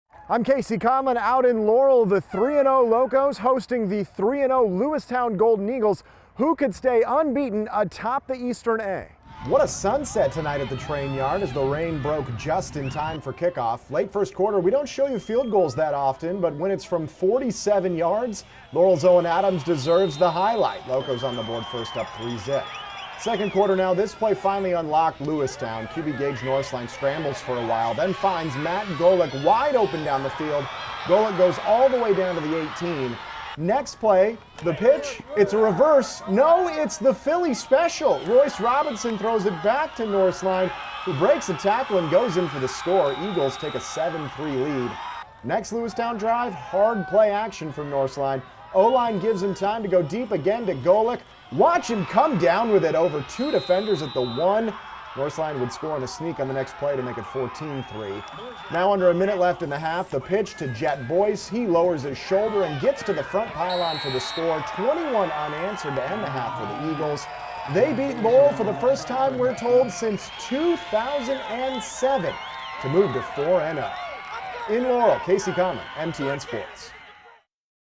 Lewistown scores 21 unanswered to beat Laurel, stay undefeated
The Lewistown Golden Eagles scored 21 unanswered points in the 2nd quarter to win a battle of Eastern A unbeatens in Laurel, their first win over the Locos since 2007.